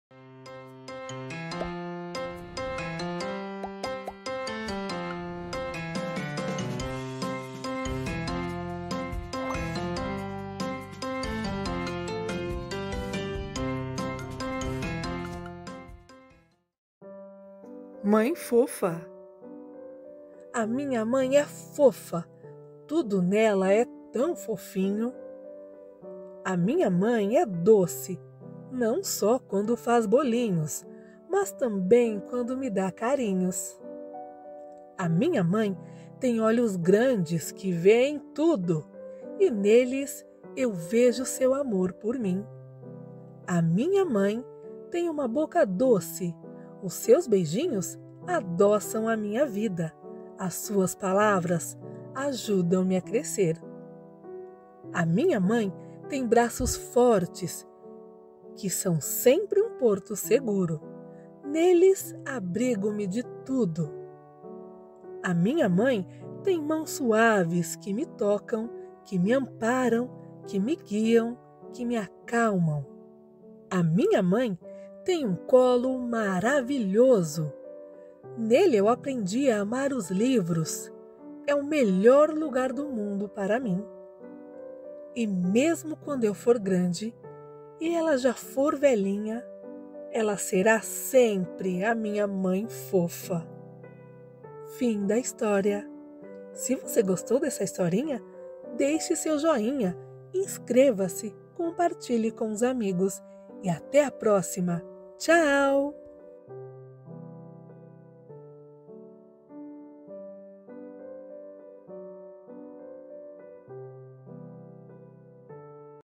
LEITURA - MÃE FOFA
Mãe fofa

A Minha MÃE é a melhor. Tenho a certeza que, para ti, a Tua MÃE também é a melhor. Para todos nós, a Nossa MÃE é única, especial e a mais querida de todas. Mãe fofa é uma homenagem à minha, à tua e a TODAS as MÃES deste mundo.

Autor desconhecido

Arrume um cantinho especial para ouvir e curtir essa historinha! Vai ser joinha! 👍🏼

👩‍🏫PROFESSORES:  Os nossos vídeos podem ser utilizados para fins EDUCACIONAIS desde que seja utilizado o link original do YouTube em respeito aos produtores de conteúdo.
Gratidão❣